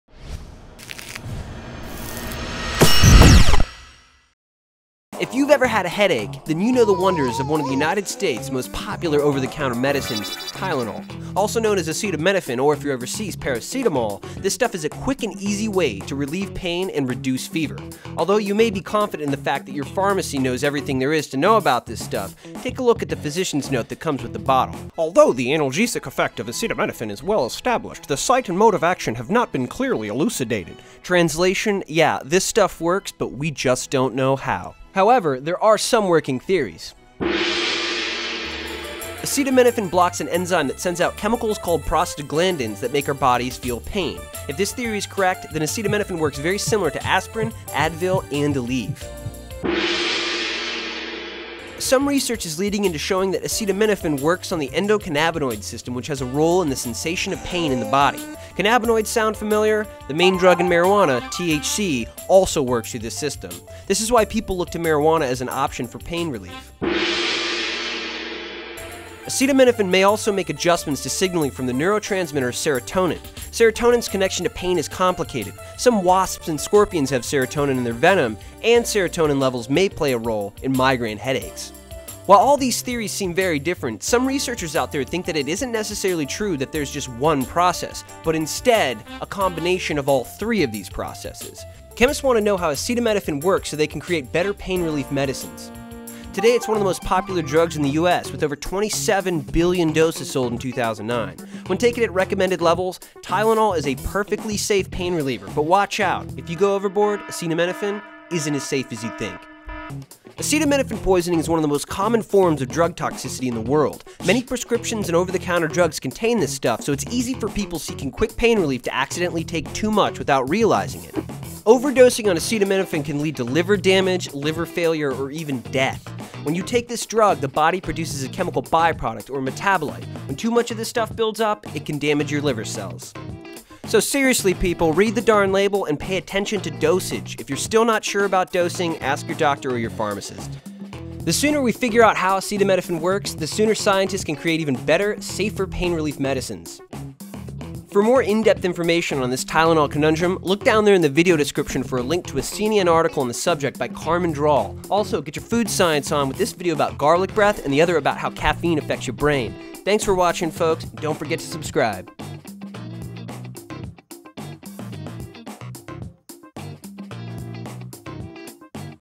How Does Tylenol Work? The truth Is--We Don't Know
This week Reactions is looking at the science and chemistry behind Tylenol.

Acetaminophen, the active ingredient in Tylenol™, is one of the most popular pain relievers in the world, selling more than 27 billion doses in 2009 alone. It can reduce fevers, eliminate aches and pains and relieve cough and cold symptoms. But how does it work? The truth is, no one knows exactly. This week, Reactions examines the theories about the popular pill.

Special thanks to C&EN -- find out more about this story

Leave your episode ideas in the comments!

Tylenol is a registered trademark of McNeil Consumer Healthcare